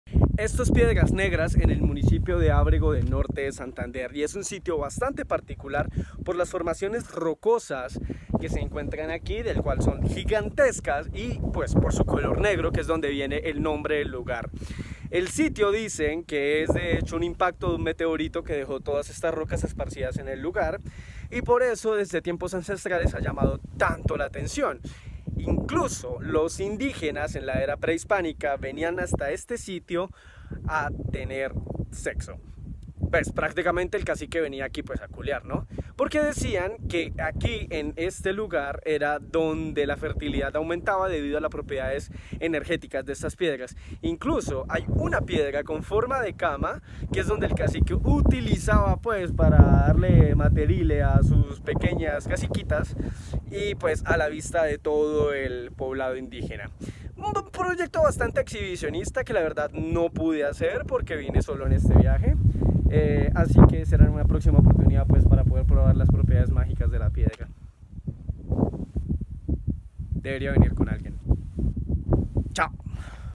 Historia 17 - Piedras Negras: donde el placer se vuelve ritual.
Se dice que en este lugar cayó un meteorito. Sea cierto o no, los indígenas se lo agarraron pa'otras vainas. Esto es Ábrego, Norte de Santander y este es el lugar donde desde hace cientos de años los indígenas Nutibara venían a tener rutios sexuales.